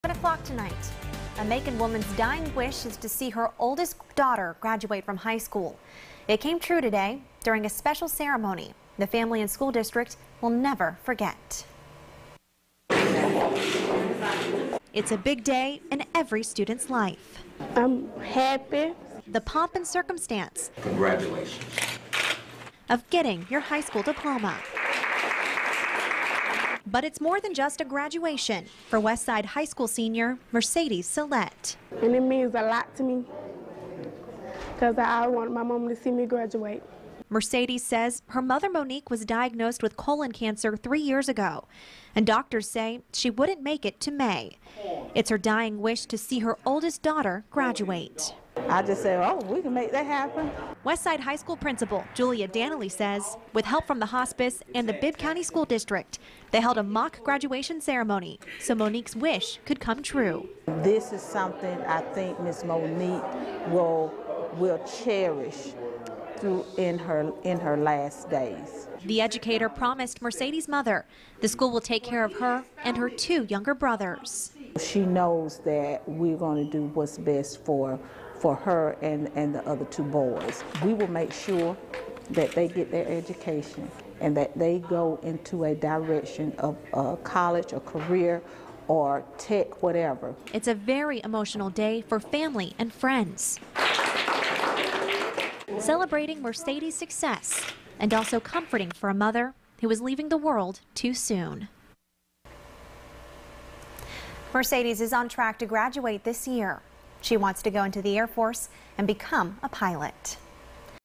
Macon student "graduates" as part of mother's dying wish
A Macon woman's dying wish is to see her oldest daughter graduate from high school.